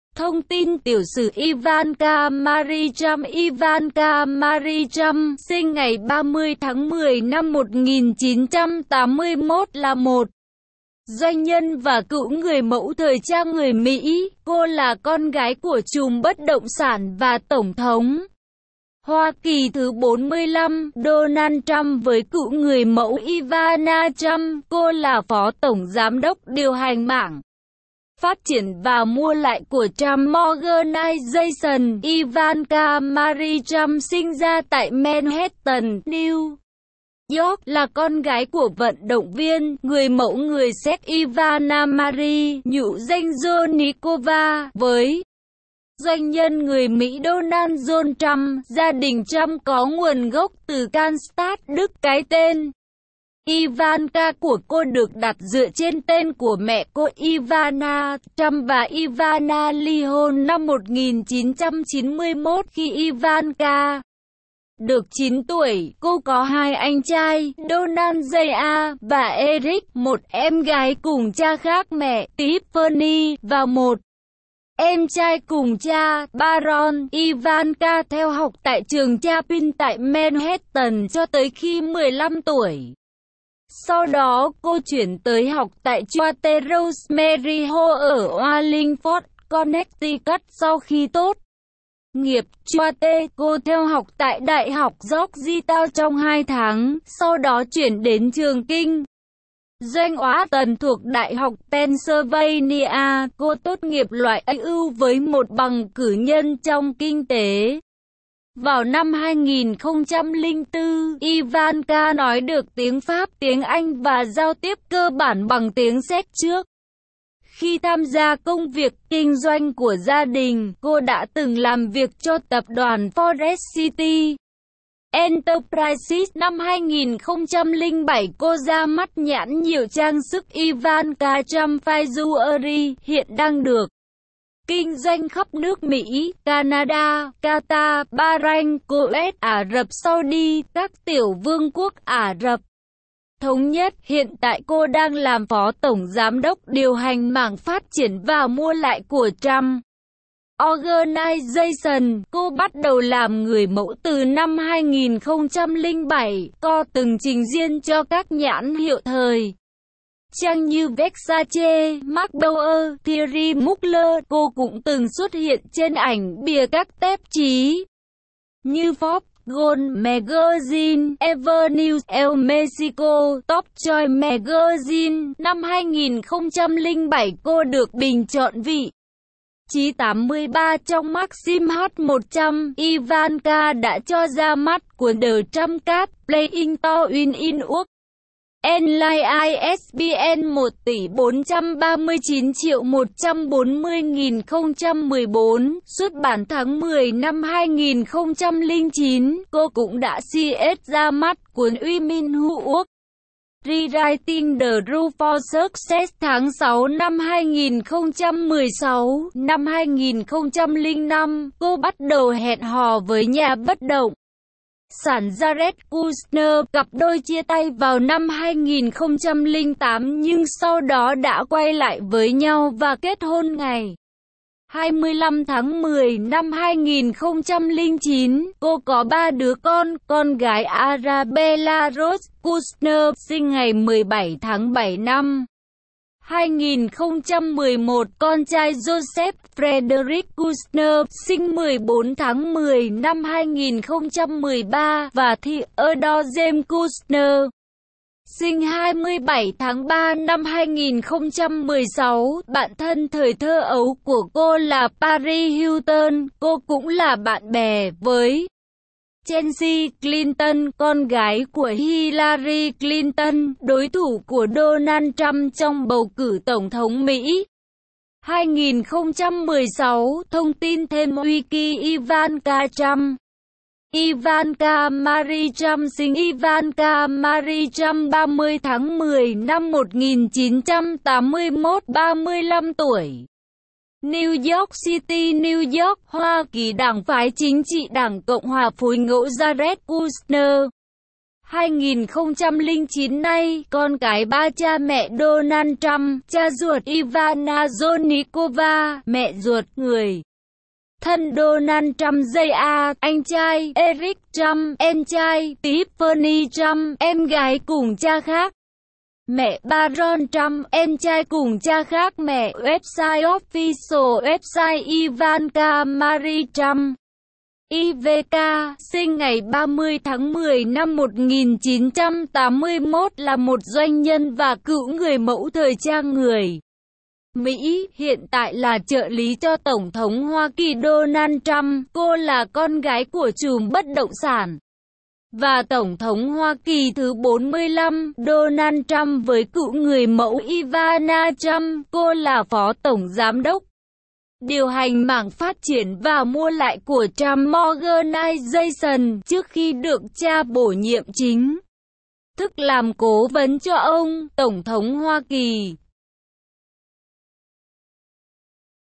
Tieu su Ivanka Trump
Thong tin tieu su Ivanka Marie Trump  Ivanka Marie Trump (sinh ngay 30 thang 10 nam 1981) la mot doanh nhan va cuu nguoi mau thoi trang nguoi My. Co la con gai cua trum bat dong san va Tong thong Hoa Ky thu 45, Donald Trump voi cuu nguoi mau Ivana Trump. Co la pho tong giam doc dieu hanh mang phat trien va mua lai cua Trump Organization.   Ivanka Marie Trump sinh ra tai Manhattan, New York, la con gai cua van dong vien, nguoi mau nguoi Sec Ivana Marie (nhu danh Zelnickova)  voi doanh nhan nguoi My Donald John Trump. Gia dinh Trump co nguon goc tu Kallstadt, Duc. Cai ten Ivanka cua co duoc dat dua tren ten cua me co Ivana. Trump va Ivana ly hon nam 1991, khi Ivanka duoc 9 tuoi. Co co 2 anh trai, Donald Jr. va  Eric; mot em gai cung cha khac me, Tiffany; va mot em trai cung cha, Barron.  Ivanka theo hoc tai truong Chapin tai Manhattan cho toi khi 15 tuoi, sau do co chuyen toi hoc tai Choate Rosemary Hall o Wallingford, Connecticut.  Sau khi tot nghiep Choate, co theo hoc tai Dai hoc Georgetown trong hai thang, sau do chuyen den Truong kinh Doanh Wharton thuoc dai hoc Pennsylvania, co tot nghiep loai uu voi mot bang cu nhan trong kinh te vao nam 2004. Ivanka noi duoc tieng Phap, tieng Anh va giao tiep co ban bang tieng Sec.  Truoc khi tham gia cong viec kinh doanh cua gia dinh, co da tung lam viec cho Tap doan Forest City Enterprises. Nam 2007 co ra mat nhan nhieu trang suc Ivanka Trump Fine Jewelry, hien dang duoc kinh doanh khap nuoc My, Canada, Qatar, Ba-ranh, Co-oet,A rap Saudi, Cac tieu vuong quoc A rap thong nhat. Hien tai co dang lam Pho Tong giam doc dieu hanh mang Phat trien va Mua lai cua Trump Organization.  Co bat dau lam nguoi mau tu nam 2007, co tung trinh dien cho cac nhan hieu thoi trang nhu Versace, Marc Bouwer, Thierry Mugler. Co cung tung xuat hien tren anh bia cac tap chi nhu Forbes, Golf Magazine, Avenue, Elle Mexico, Top Choice Magazine. Nam 2007 co duoc binh chon vi tri 83 trong Maxim hot 100.  Ivanka da cho ra mat cuon The Trump Card: Playing to Win in Work and Life ISBN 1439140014, xuat ban thang 10 nam 2009. Co cung da ch ra mat cuon  Women Who Work: Rewriting the Rules for Success thang 6 nam 2016.  Nam 2005, co bat dau hen ho voi nha bat dong san Jared Kushner. Cap doi chia tay vao nam 2008 nhung sau do da quay lai voi nhau va ket hon ngay 25 thang 10 nam 2009. Co co 3 dua con, con gai  Arabella Rose Kushner (sinh ngay 17 thang 7 nam 2011) con trai Joseph Frederick Kushner (sinh 14 thang 10 nam 2013) va Theodore James Kushner (sinh 27 thang 3 nam 2016).  Ban than thoi tho au cua co la Paris Hilton. Co cung la ban be voi Chelsea Clinton (con gai cua Hillary Clinton, doi thu cua Donald Trump trong bau cu Tong thong My 2016).                                                        Thong tin them Wiki Ivanka Trump                  Ivanka Marie Trump   Sinh  Ivanka Marie Trump 30 thang 10, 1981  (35 tuoi) New York City New York, Hoa Ky  Dang phai chinh tri  Dang Cong Hoa  Phoi ngau  Jared Kushner  (2009nay)  Con cai  3  Cha me  Donald Trump  (cha ruot) Ivana Zelnickova  (me ruot)  Nguoi than  Donald Trump Jr.  (anh trai) Eric Trump  (em trai) Tiffany Trump  (em gai cung cha khac me) Barron Trump  (em trai cung cha khac me)  Website  Official website  Ivanka Marie Trump  (/ivk/, sinh ngay 30 thang 10 nam 1981) la mot doanh nhan va cuu nguoi mau thoi trang nguoi My, hien tai la tro ly cho Tong thong Hoa Ky Donald Trump.  Co la con gai cua trum bat dong san va Tong thong Hoa Ky thu 45, Donald Trump voi cuu nguoi mau Ivana Trump. Co la pho tong giam doc dieu hanh mang phat trien va mua lai cua Trump Organization. truoc khi duoc cha bo nhiem chinh thuc lam co van cho ong (tong thong Hoa Ky).